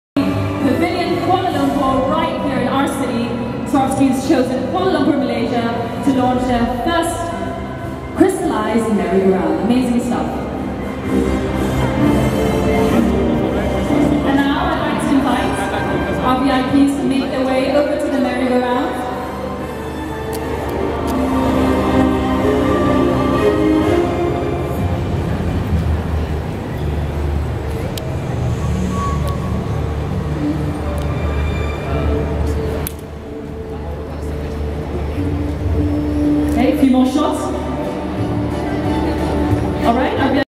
The Swarovski Merry Go Round in Pavillion KL
Are you in the momento of Christmas and Christmas tree is everywhere! And this year, Pavilion brings you with their first and the world first Swarovski Crystallised Merry-Go-Round at Pavilion Kuala Lumpur, Malaysia’s premier shopping destination.